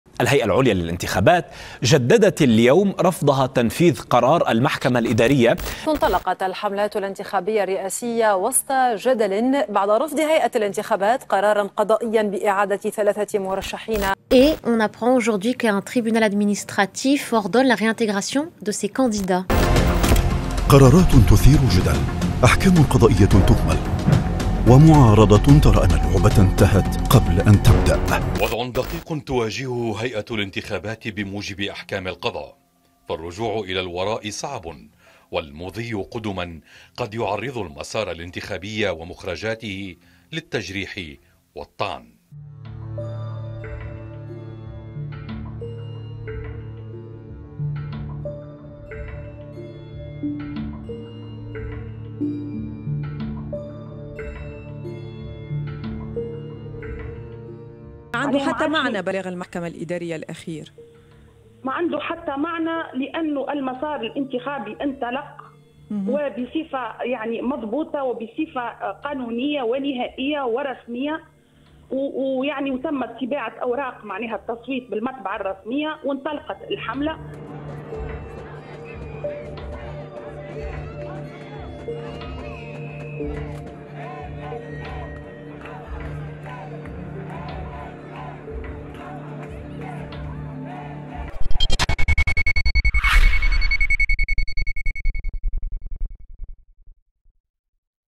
نواة في دقيقة: تونس كلها تعرف، لكنها تحبس أنفاسها
عندما تتحول الأحكام القضائية الملزمة إلى جدل ومجرد رأي مهمل، كيف يمكن اقناع المواطنين باحترام حكم القضاء ولهم في صاحبة الولاية الكاملة الأسوة الحسنة. ذلك هو حال الانتخابات الرئاسية فيما تبقى من أيام قبل موعد 6 أكتوبر.